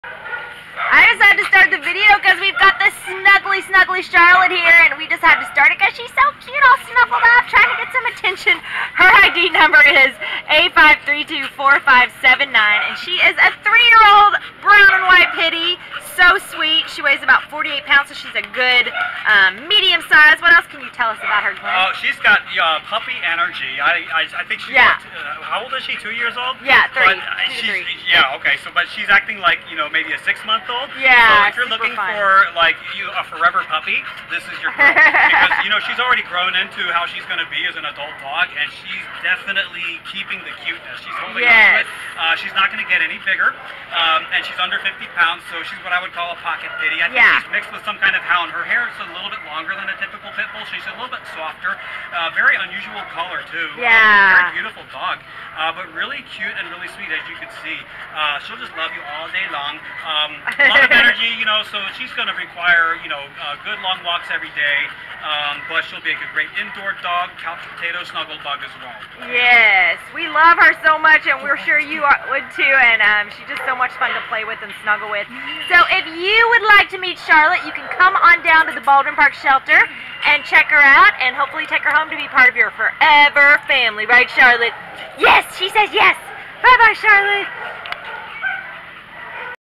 A5324579 Charlotte | Pit Bull
A5324579 Charlotte is a lovely 3-year-old chocolate-with-white female Pit Bull who came  to the Baldwin Park Animal Care Center on October 5th as a stray. Weighing 48 lbs, Charlotte is a sweetheart! She’s good on leash, seems to be housebroken, and is good with other dogs. She has a real puppy personality, snuggly and playful -- she’s happy to explore or give kisses to all her new friends. Charlotte has a beautiful coat, cute little forehead wrinkles, and a nonstop smile. She has a little hair loss near her tail which should be seen by an outside vet, but will probably clear up with good-quality food. Charlotte is going to make a wonderful buddy -- she’s got grown-up manners and a sweet, charming puppy personality. On the smaller side for her breed, she’s a born lap pup!